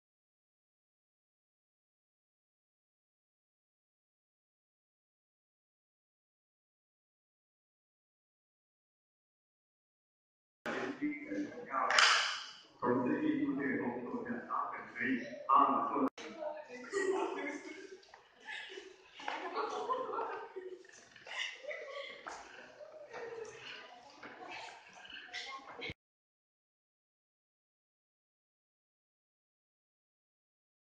好姊妹 - 一起走吧！Sistergood –For a Walk 2015, mixed media, 100*40*165cm
Delusional future with her.
Keep her in a stable state.
But to others, it is just a weird cart.

《The Store of Contemporary Art in Tainan│2017 Next Art Tainan Prize》Audience participation at Licence Art Gallery, Tainan, Taiwan

Photo / Randy Yang